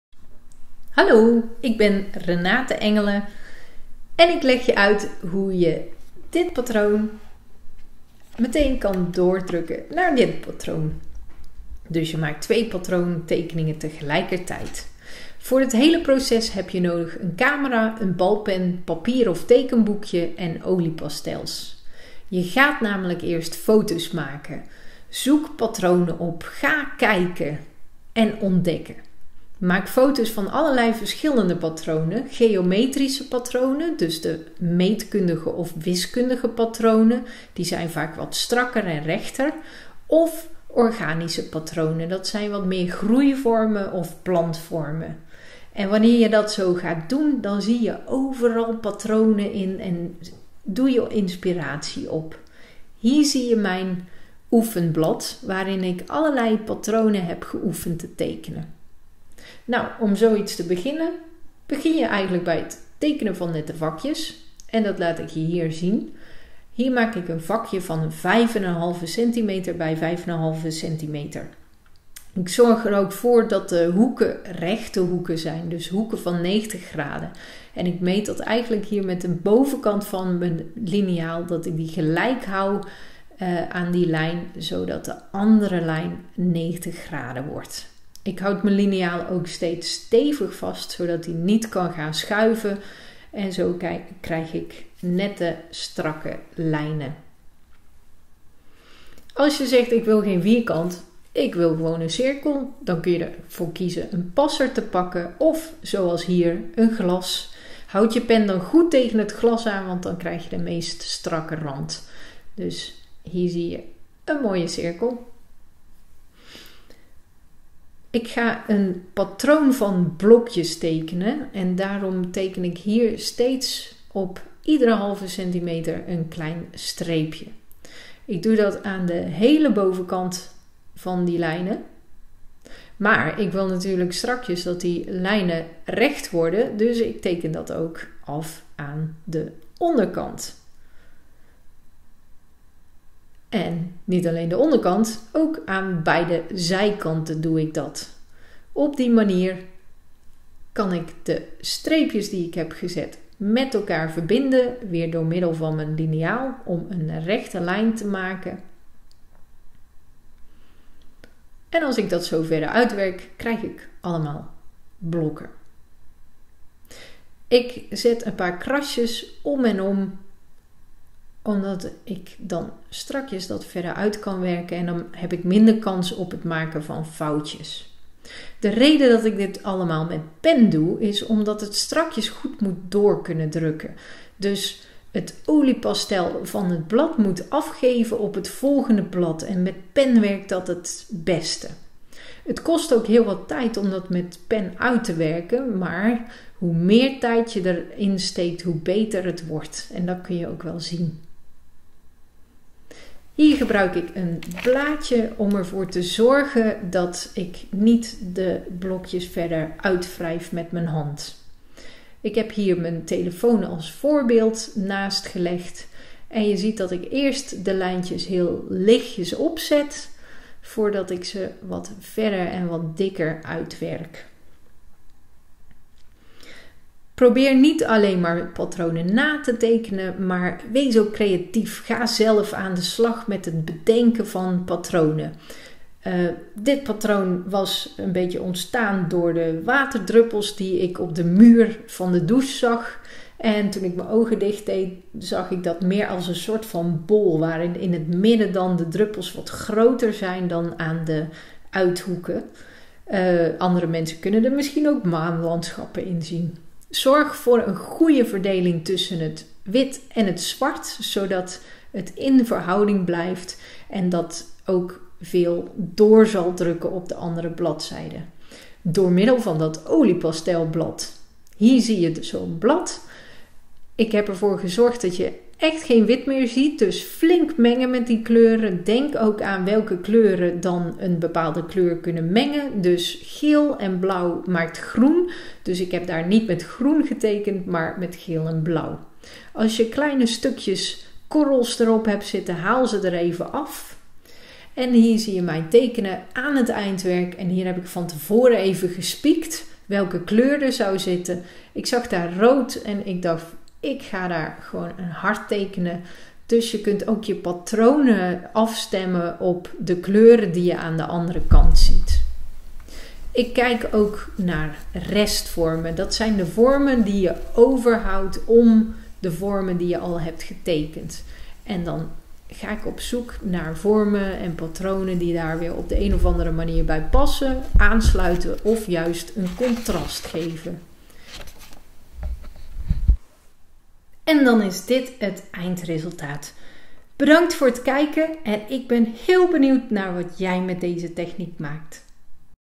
Zen patronen (+ print)
In deze video laat ik je verschillende ideeën zien om mee aan de slag te gaan:
- Het tekenen van patronen en daar direct een afdruk van maken.
- Het opdoen van inspiratie door het maken van foto's
- Het oefenen van tekenen van patronen met pen
- Het maken van een blad met oliepastel om het patroon mee door te kunnen drukken op een volgend blad

Je hebt nodig:
1. een camera
2. een (zwarte) pen
3. papier/ schetsboek
4. olie pastel krijt

Ik ben benieuwd naar wat jij maakt.